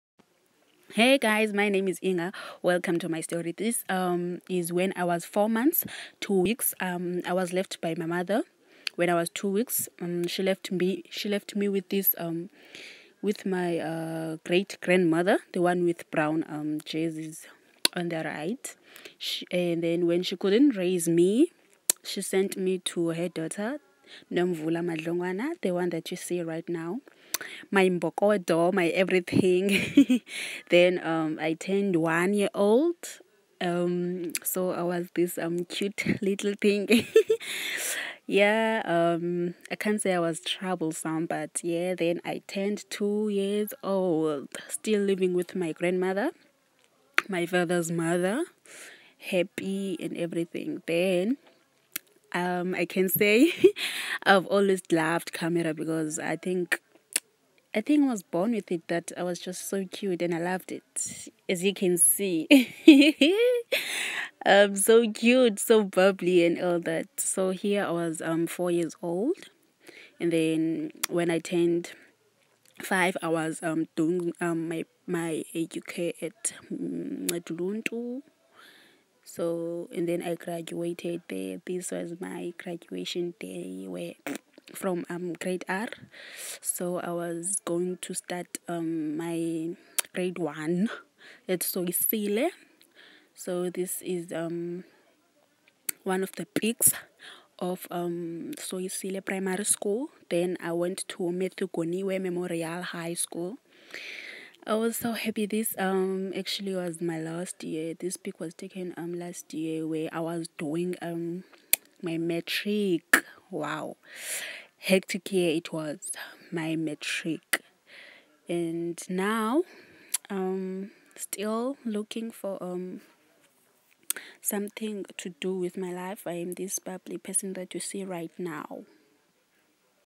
"Inga’s road to her dreams" - Until We Remember - 2020
"This shows my history, from birth to the Inga people now see. Family has always been an important part of my life and I am grateful for what they have done for me. I could not have had it any better with anyone else. I was and am still happy. I've never felt that I didn’t have a mother’s love, because I have had so many mothers in the form of aunts and a loving grandmother."